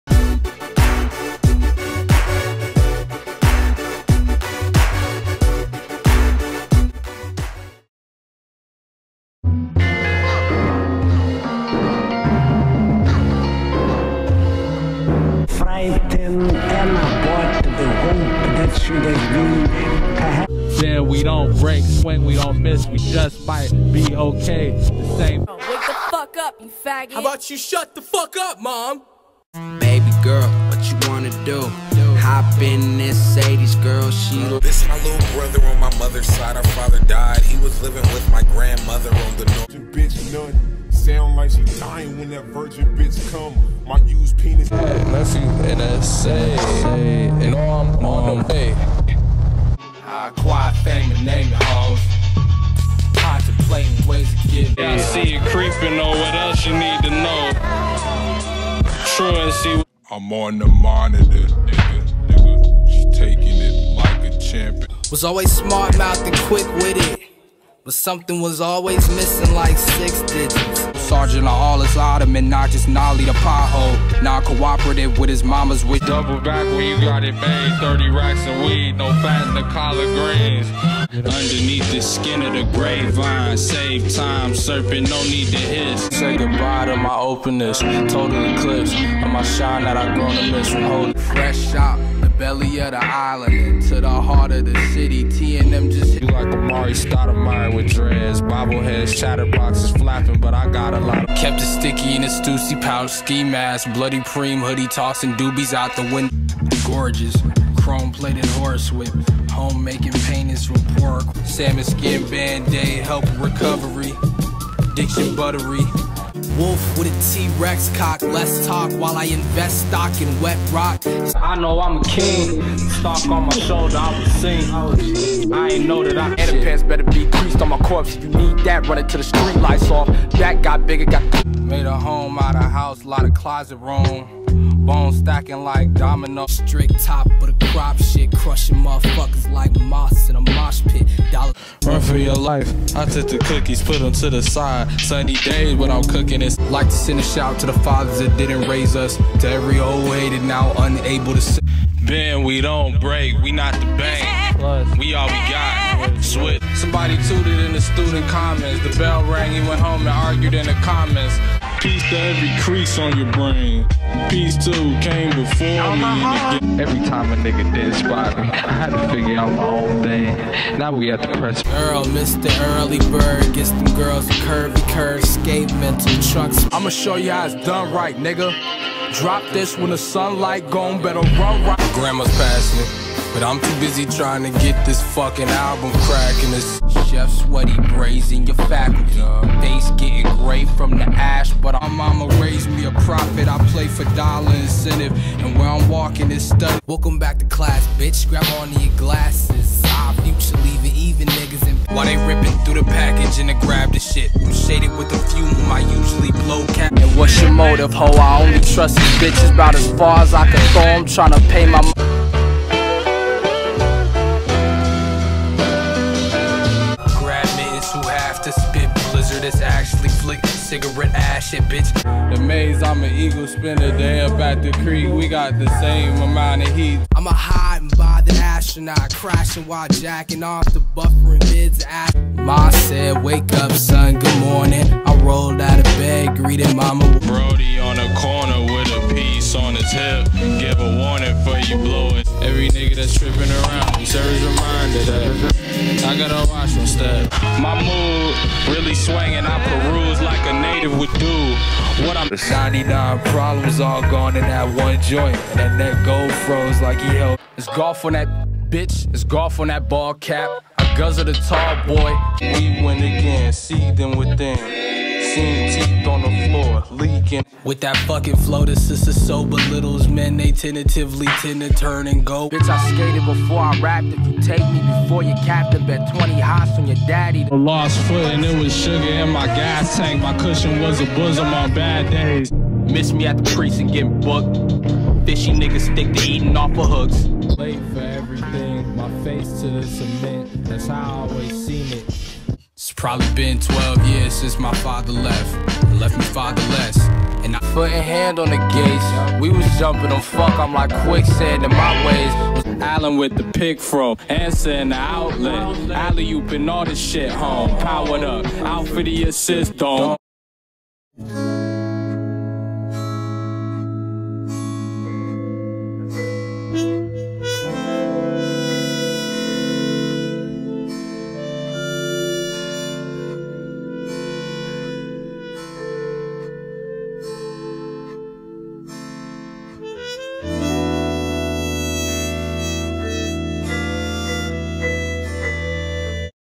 Every Earl Sweatshirt Song Ranked (2010 - 2020)
In this video, I will be ranking every song from Earl Sweatshirt from worst to best.

I put a lot of work in every video! :)